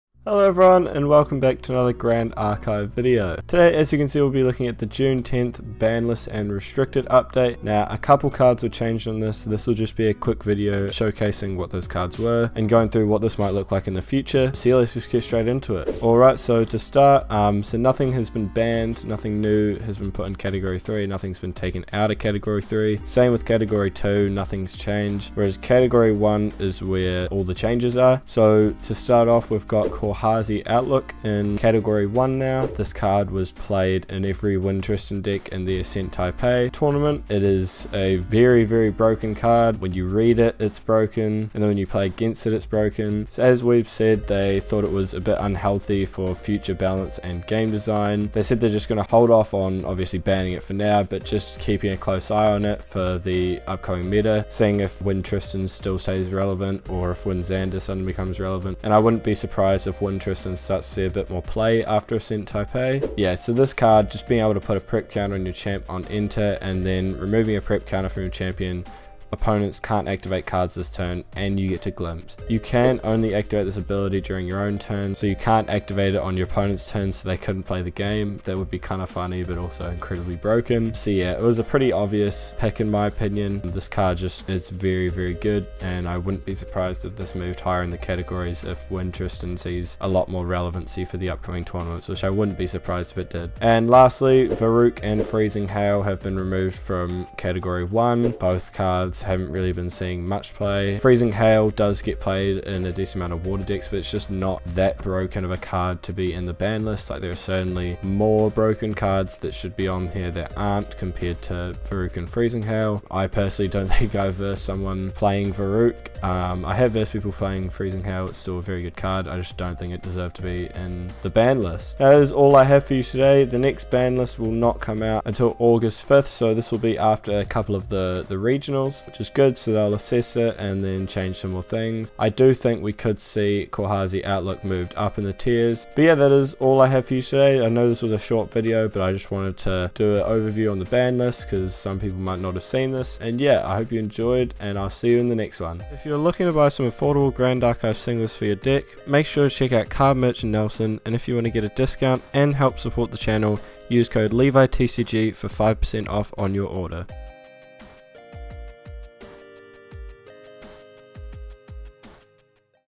Grand Archive Banlist Summary | June 10th B&R Update
Hello and welcome to another video on the LeviTCG channel! In this video I go through the new June 10th B&R update in Grand Archive! Let me know down in the comments what you think of the new changes and what you think will change in the next banlist!

Let me know down in the comments if there is any other specific content you would like to see on the channel!